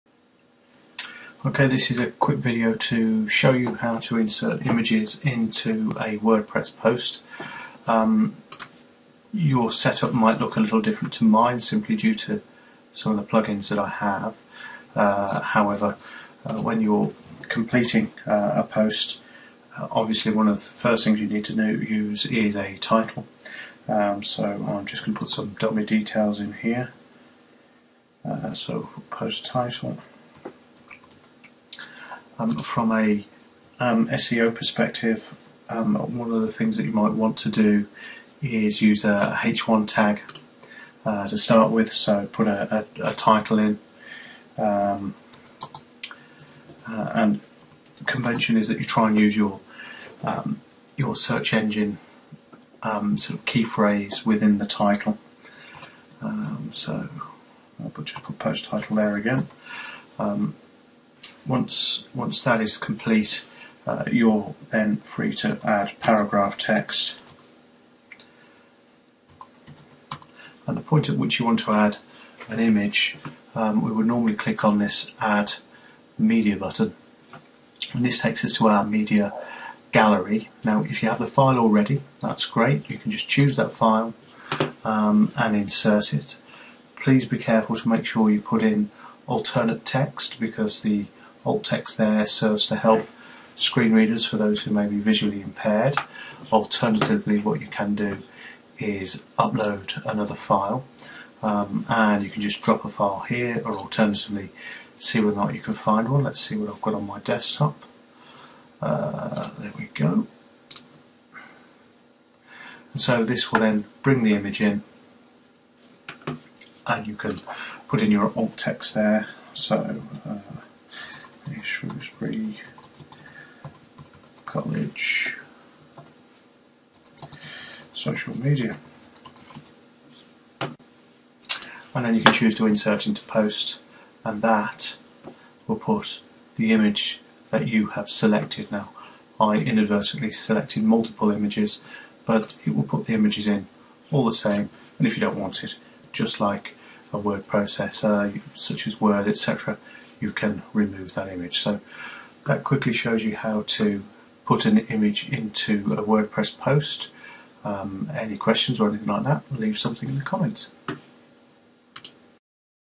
Blogging Quick Tip - Insert an Image in a WordPress post
A quick video tip showing how you can insert images into a WordPress post.